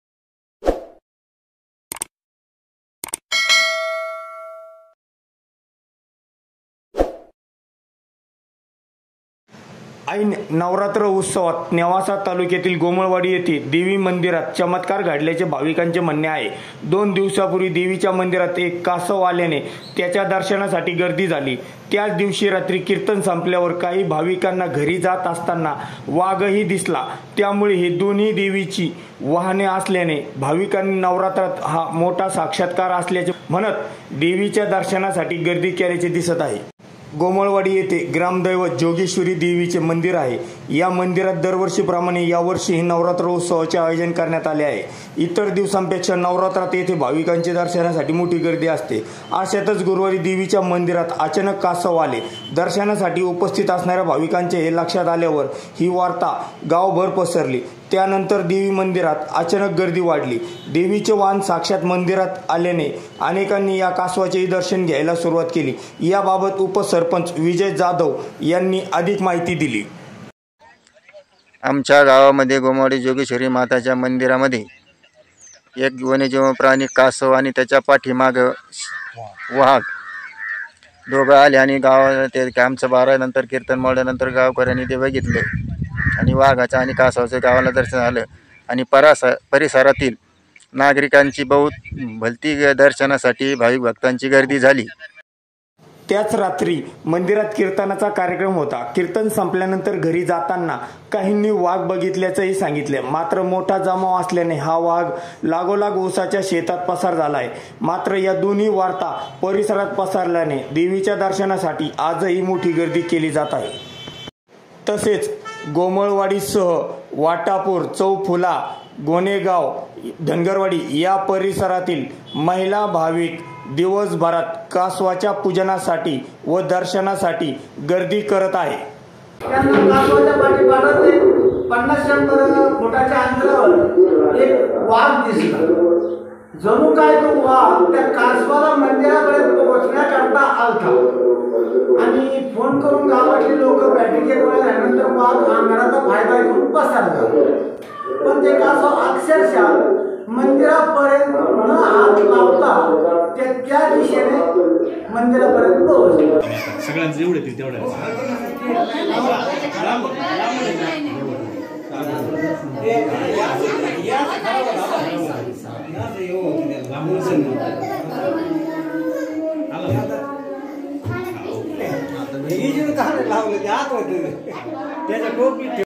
नेवासा - जोगेश्वरी मंदीरात कासव आल्याची वार्ता पसरल्याने भाविकांची गर्दी
जोगेश्वरी मंदीरात | कासव आल्याची वार्ताने | भाविकांची गर्दी | कासव व वाघ ही देवीची वाहने आल्याची चर्चा |